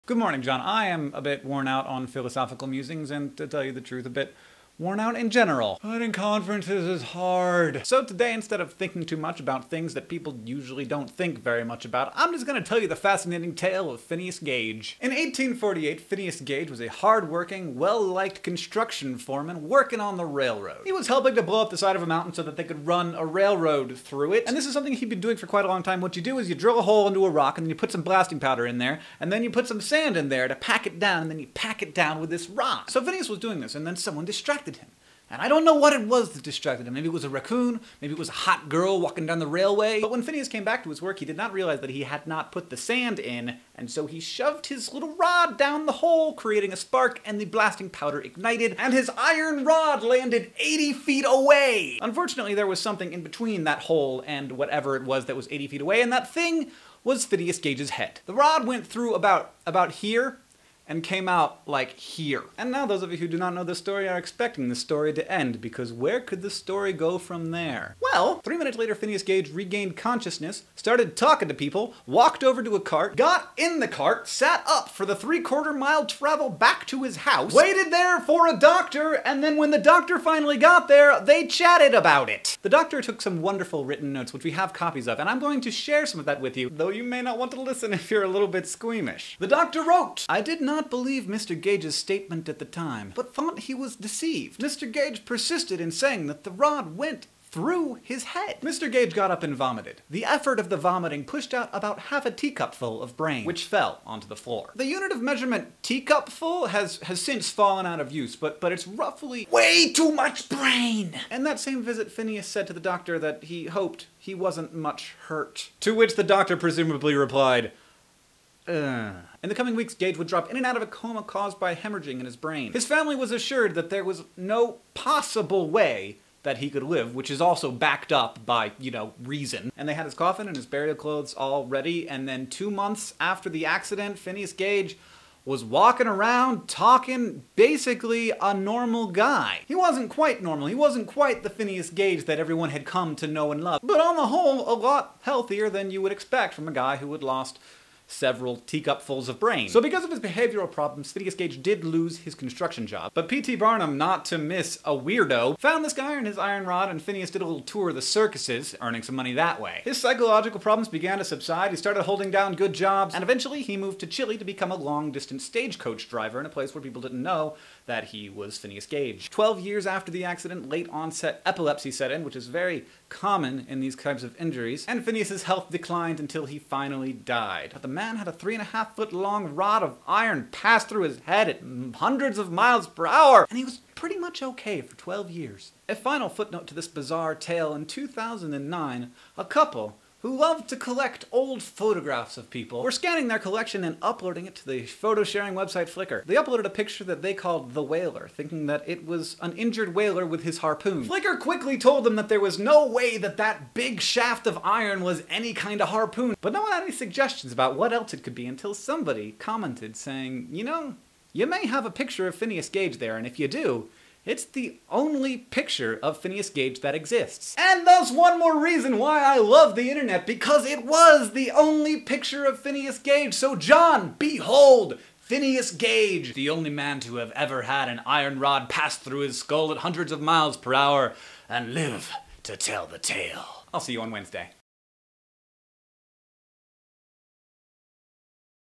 Stabbed in the Brain: Phineas Gage
The Curious Case of Phineas Gage. A railway worker in the 1800s, a iron rod passed through his head, and he recovered almost fully in a matter of months. I just wanted to tell you the story, no analysis or anything...it's just amazing, take what you want from it.